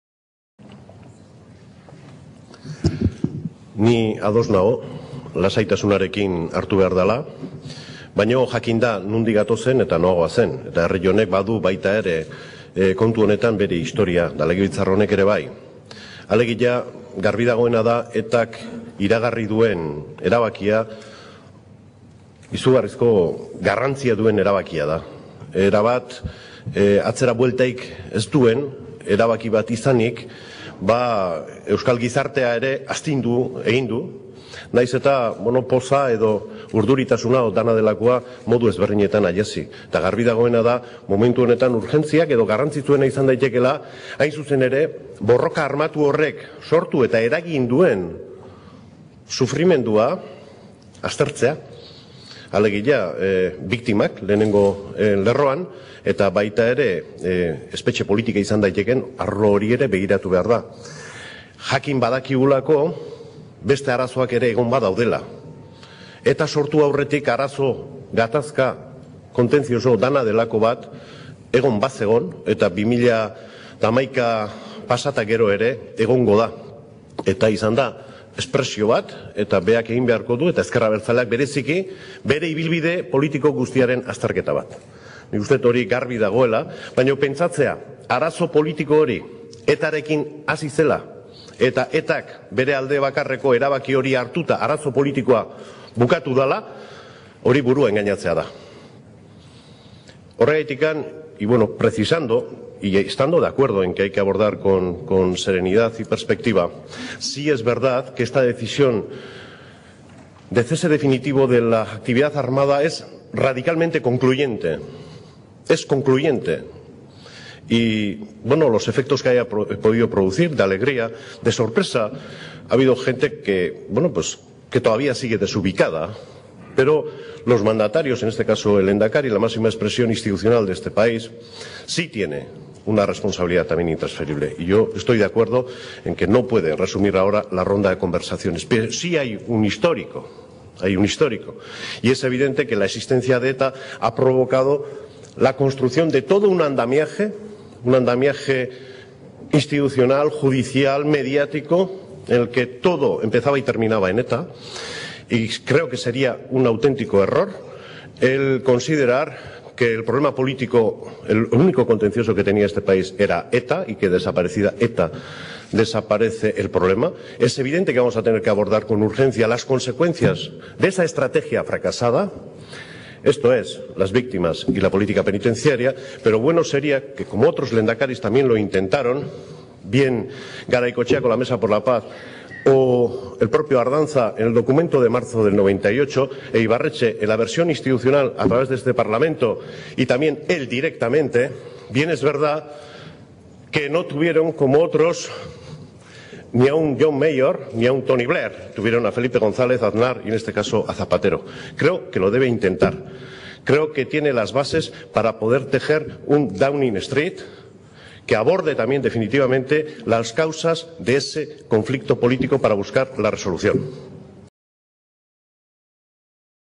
Egibar interpela a López ante el cese de actividad de ETA
Egibar pregunta a López sobre la gestión del nuevo tiempo que se abre tras la declaración de cese definitivo de la actividad armada de ETA